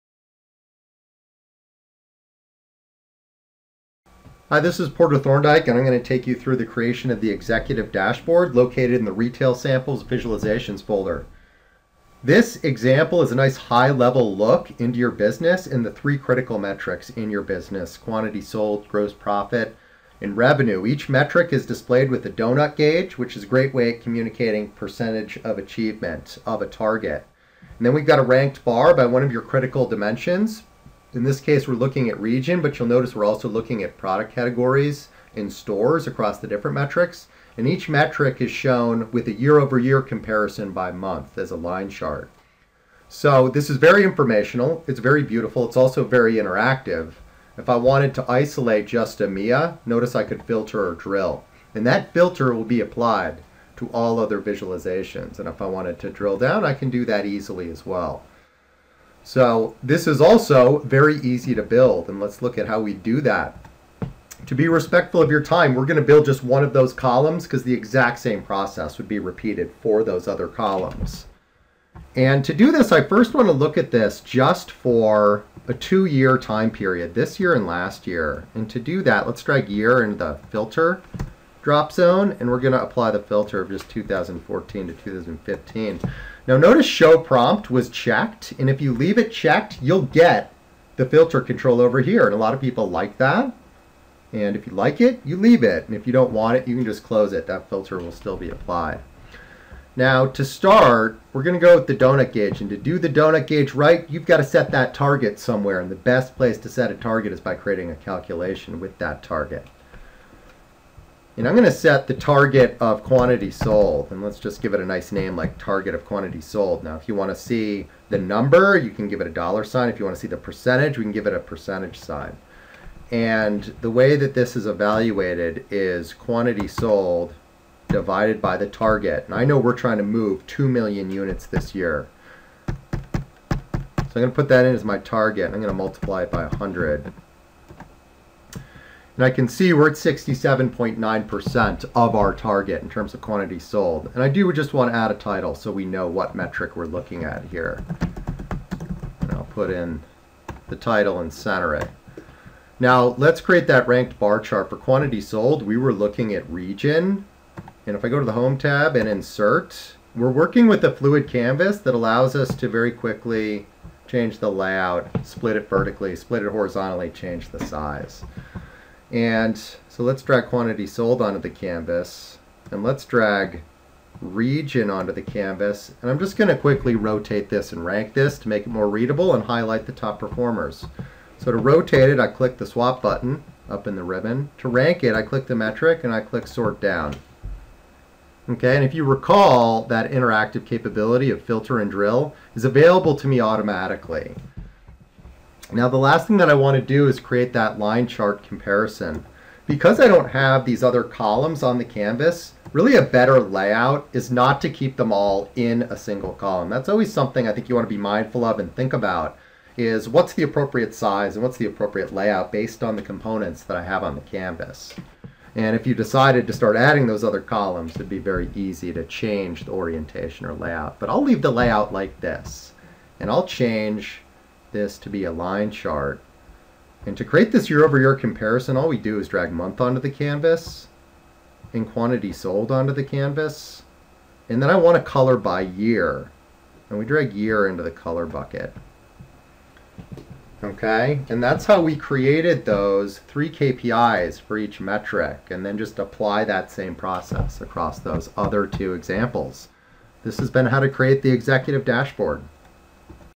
WebFOCUS 8.2 BUE Sample Visualization: Executive Dashboard
Learn how to create and modify an executive dashboard using sample  retail data.